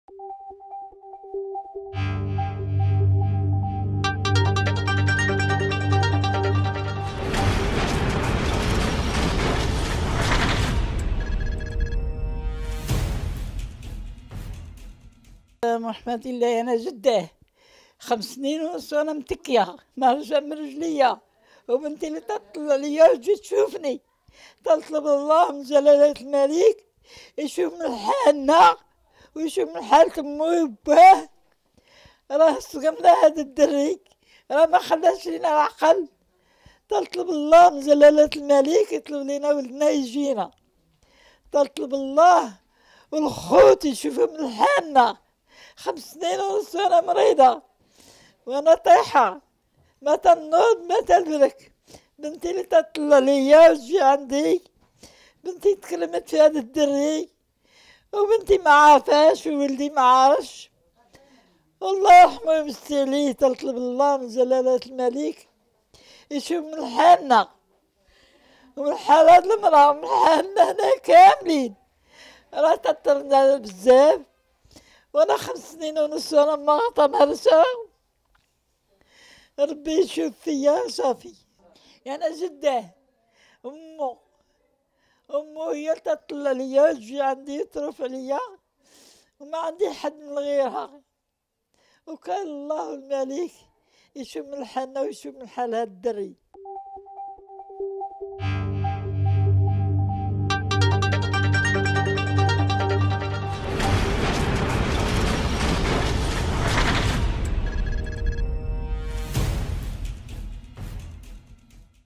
بعد الأم .. صرخة جدة محروقة على مصير الشاب الذي غرق في فرنسا بعدما وصل إليها قادما من تركيا
بعد الأم .. صرخة جدة محروقة على مصير الشاب الذي غرق في فرنسا بعدما وصل إليها قادما من تركيا | شوف تيفي 2016